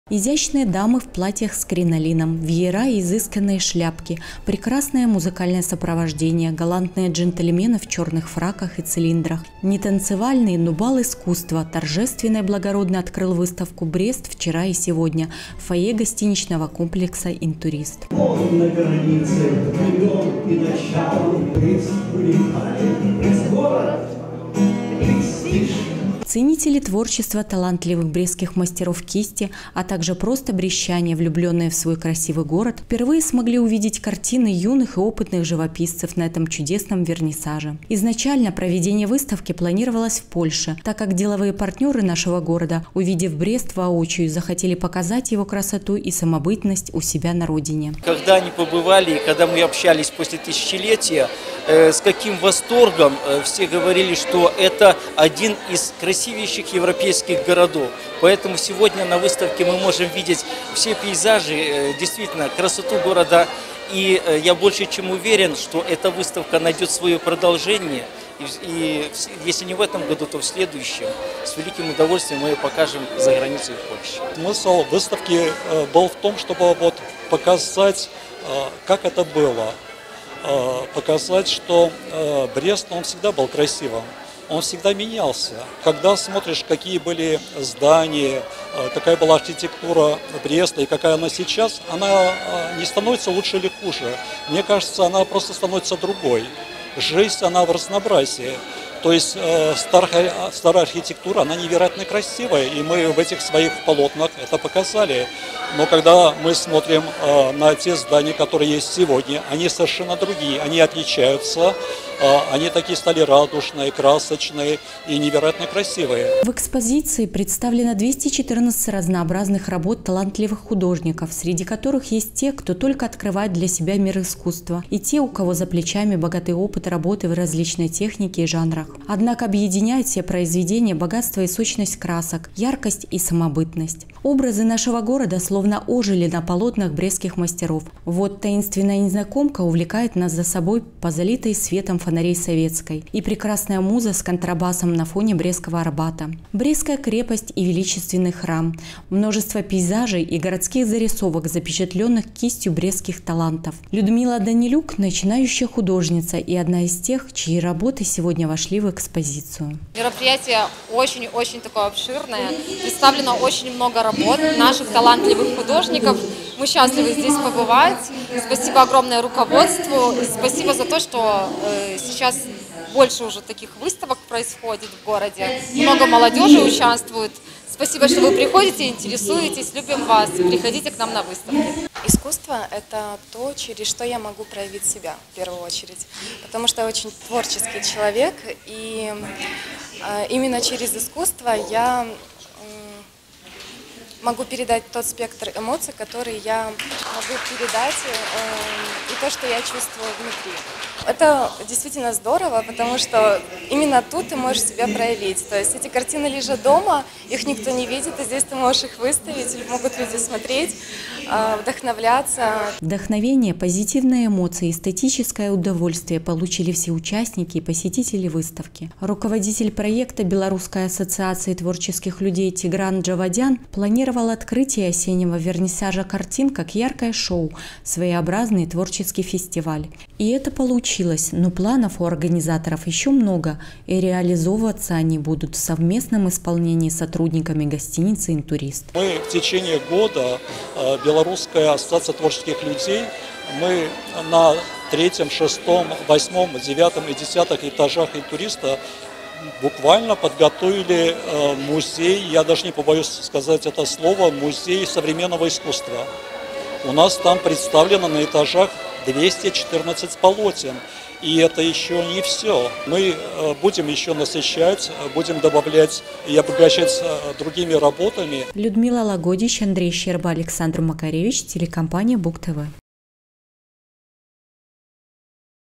2020-11-03 г. Брест. Выставка «Брест вчера и сегодня». Новости на Буг-ТВ.
Белорусская ассоциация творческих людей при поддержке администрации нашего города презентовала самую масштабную выставку этого года «Брест вчера и сегодня», приуроченную к году Малой родины. Экспозиция разместилась на пяти этажах гостиницы «Интурист», а также в фойе – всего на суд ценителей прекрасного представлено 214 произведений мастеров кисти Брестчины. Каким видят город над Бугом брестские художники - в сюжете Людмилы Лагодич.